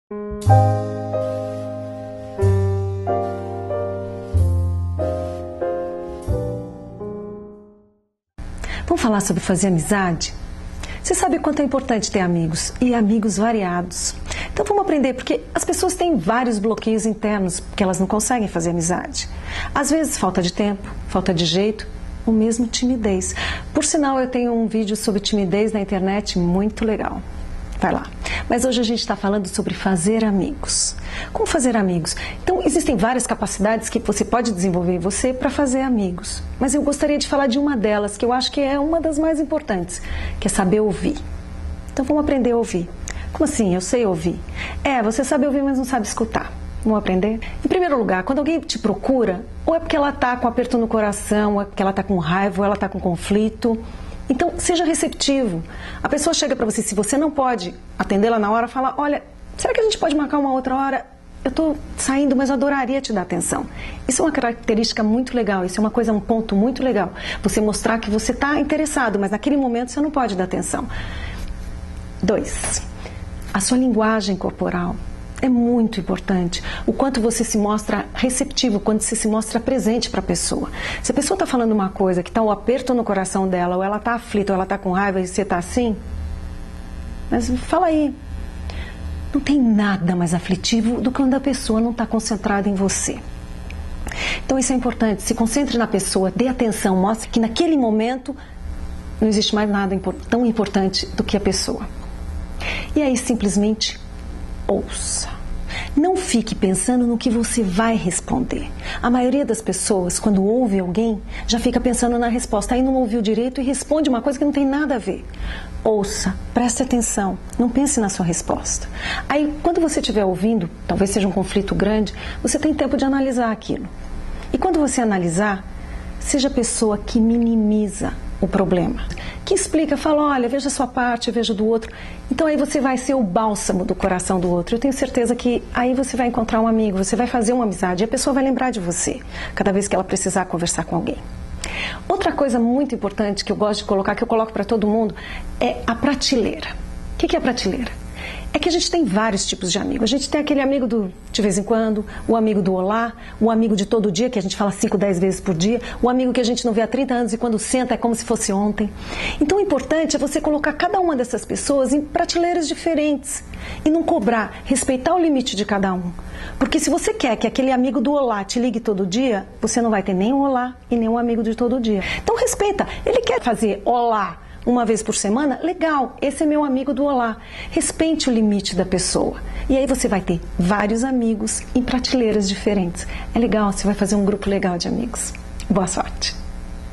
Fazer amigos
Aprenda a Fazer Amigos e entenda a Diferença entre os Tipos de Amizade.

E veja outros vídeos aqui no meu canal do youtube.

Margareth Signorelli
coach de relacionamento
certificada em EFT